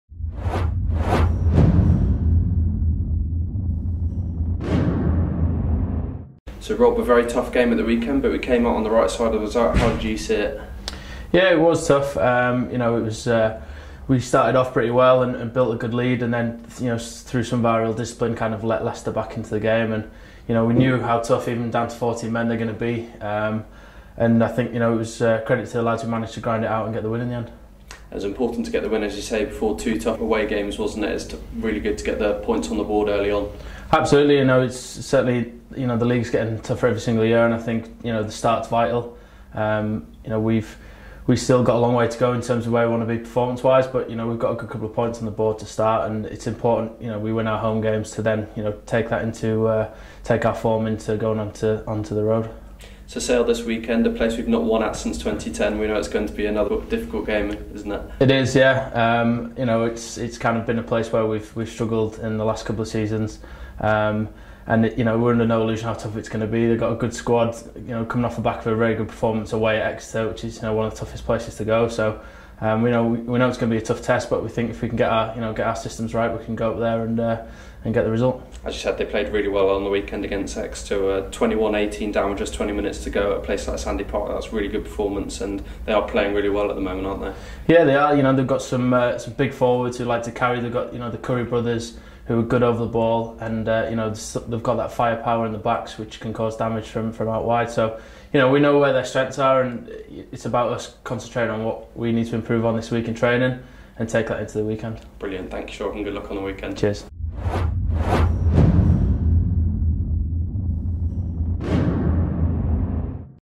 Rob Miller previews Sale Sharks
Rob Miller looks ahead to the trip to the AJ Bell Stadium to play Sale Sharks on Saturday. Don't miss Rob and the team back in action at the Ricoh Arena on Saturday 6 October against Gloucester. Get your tickets now from wasps.co.uk/tickets